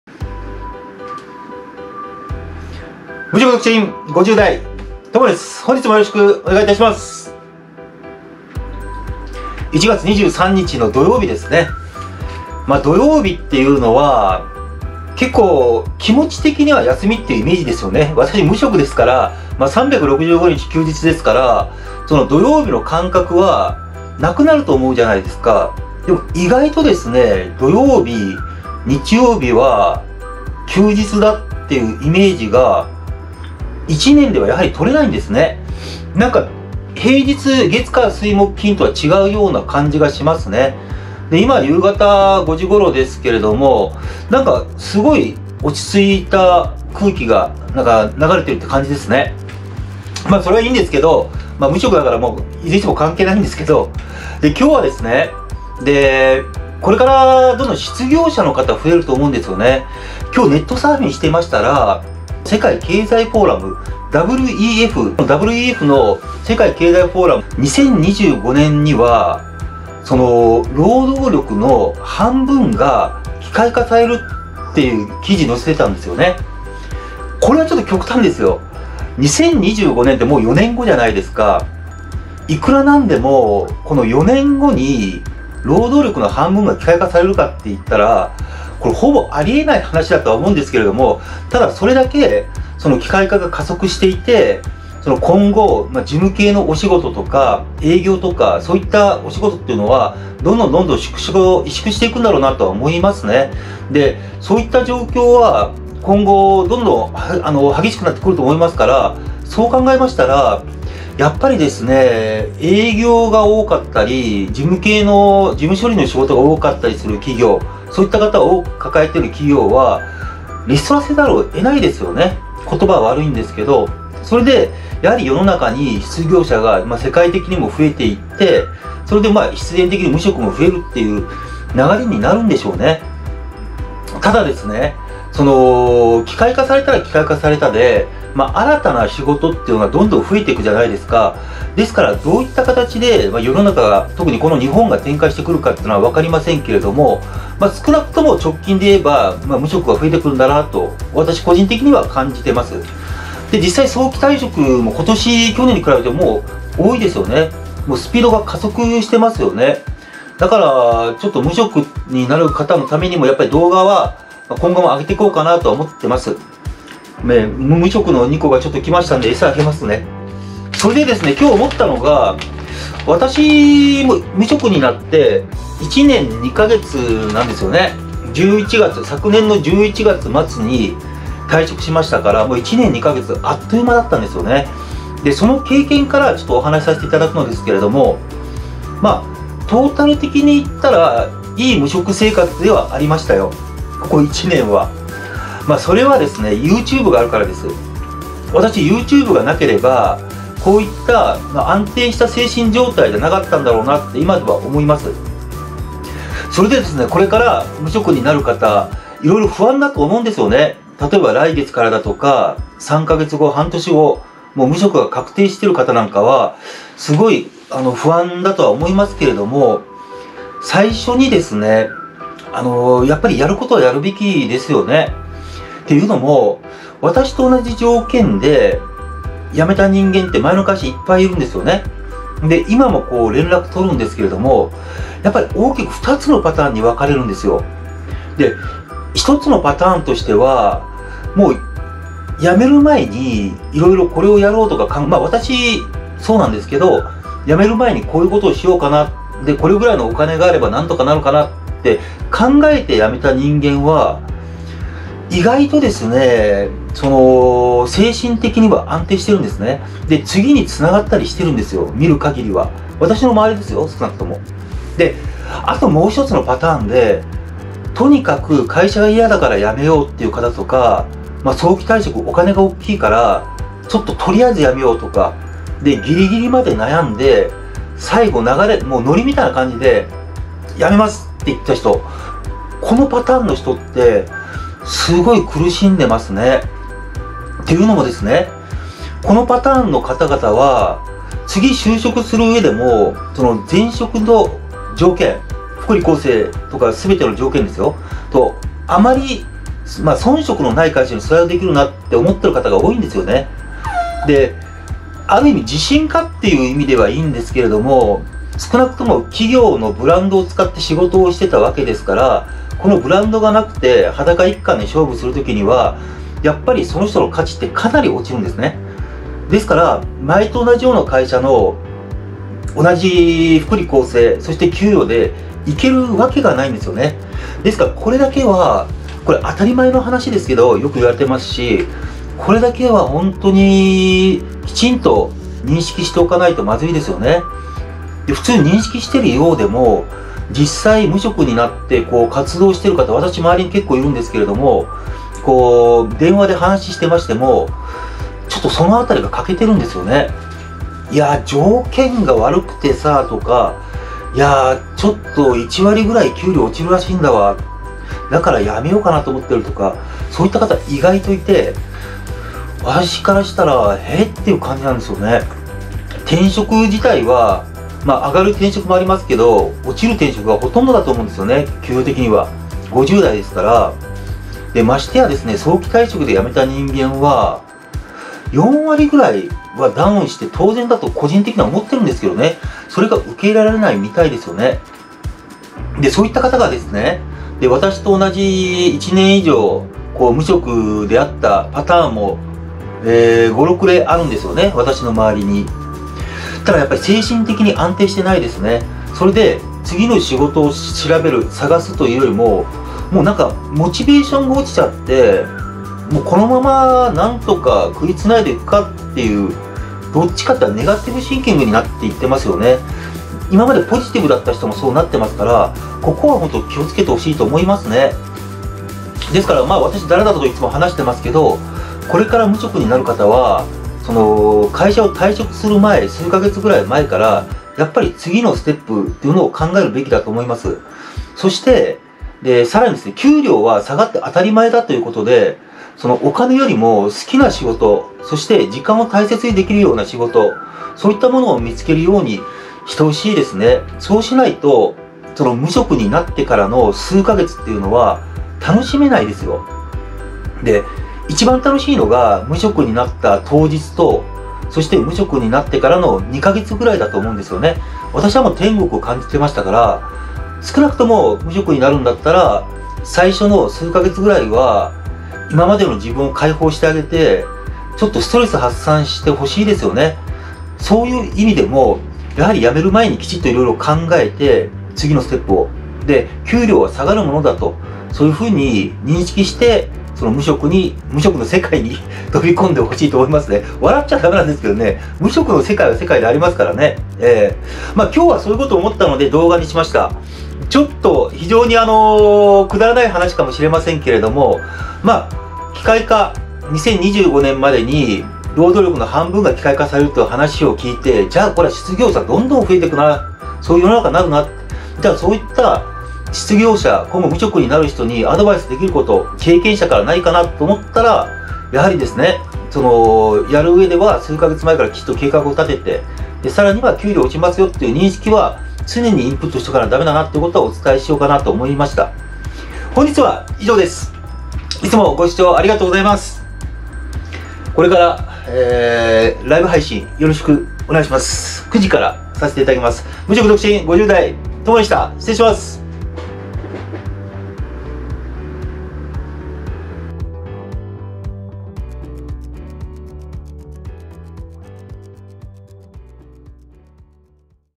無職の心得 ～考えて無職になろう！～ 無職独身50代とも
大量失業時代の到来。。。今後も無職は増え続ける。
これから無職になる方は、退職前に考えて欲しい。
本当にやめて良いのか？次はどうするのか？
私の周りの友人は、満足組と後悔組のふたパターン。
不必要なくらい慎重に。。。😎

将来が不安な面もありますが、一体どうなることやら。。。

キャンピングカー動画やってます！！